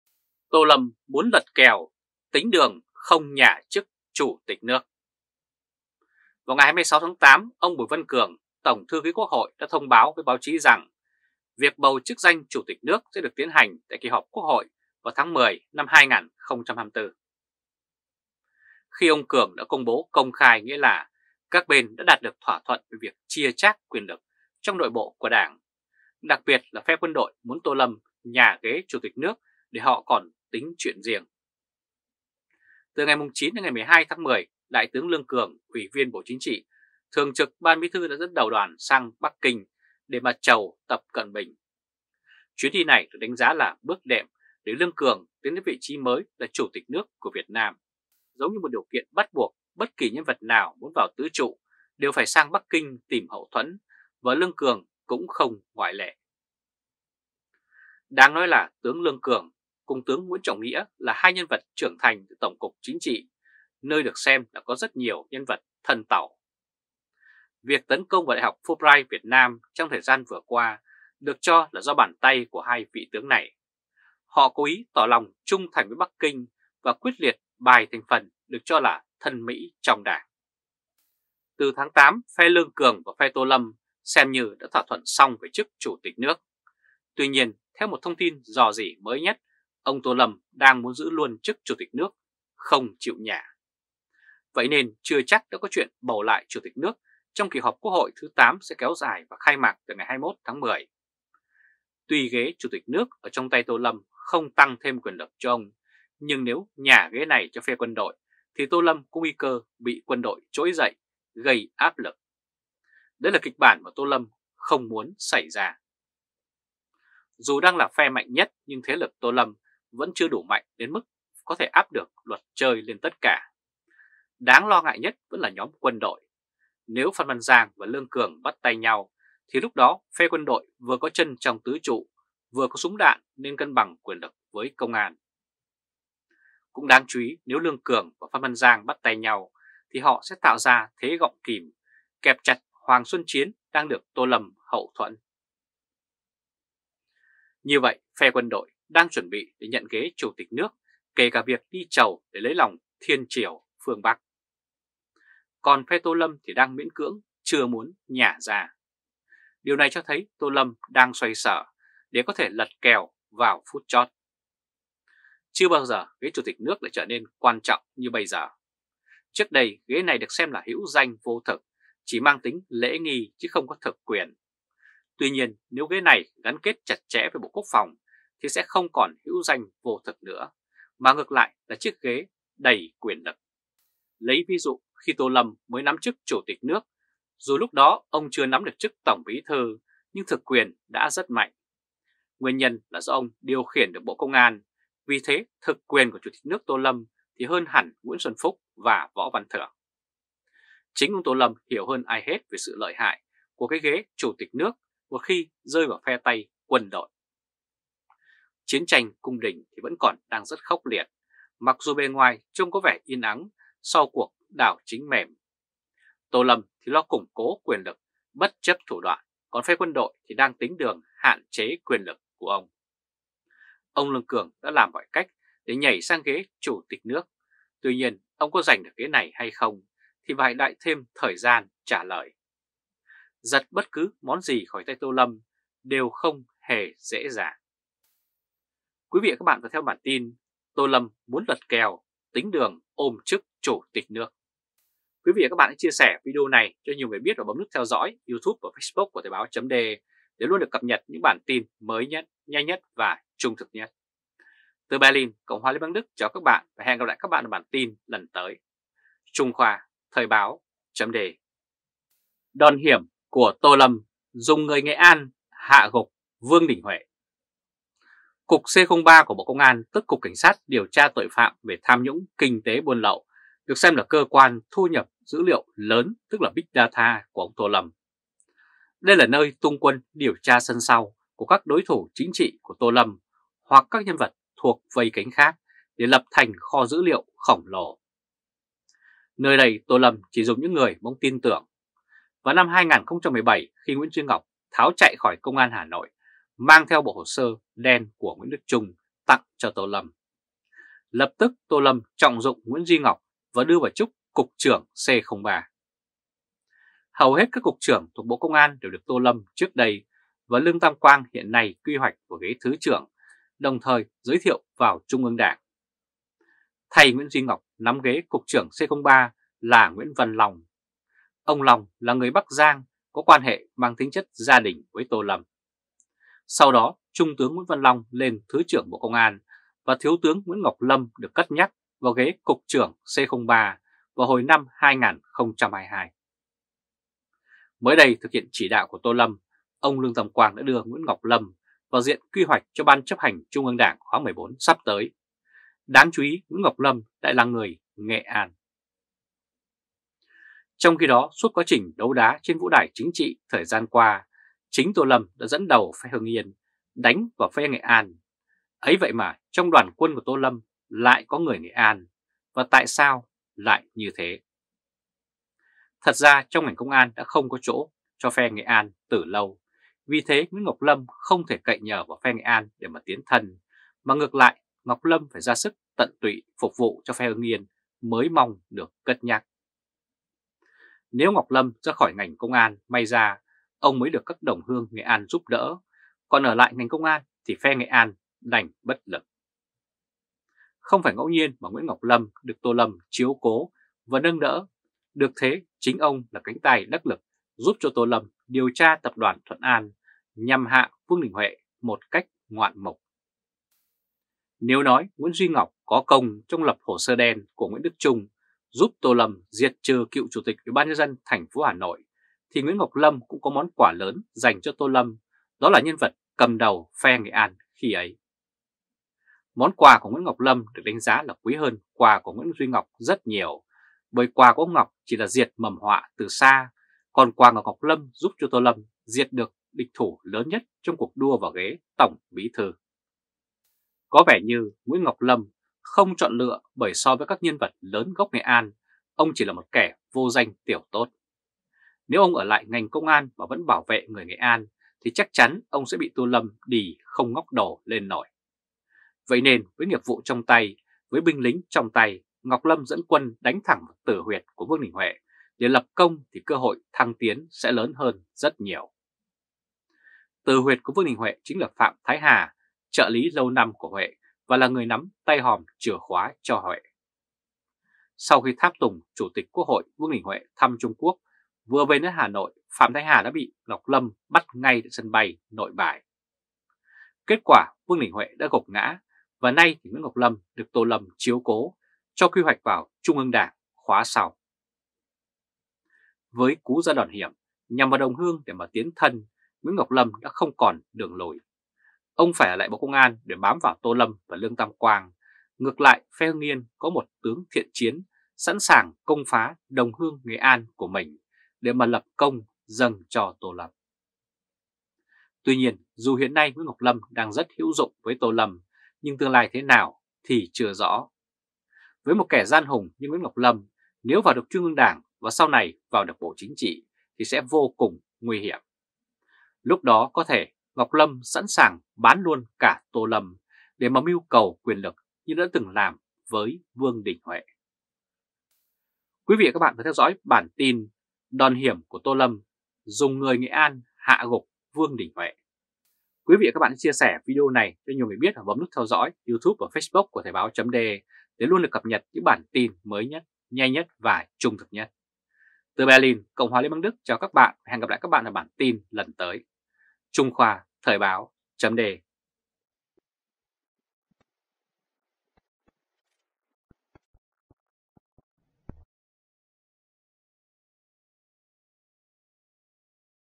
Tô Đại muốn lật kèo, tính đường ôm chức chủ tịch nước?
Ông Lương Cường đã làm mọi cách để nhảy sang ghế Chủ tịch nước. Tuy nhiên, ông có giành được ghế này hay không, thì phải đợi thời gian trả lời. Giật bất cứ món gì khỏi tay Tô Lâm đều không hề dễ dàng.

Xem bài chi tiết tại:
Hoặc: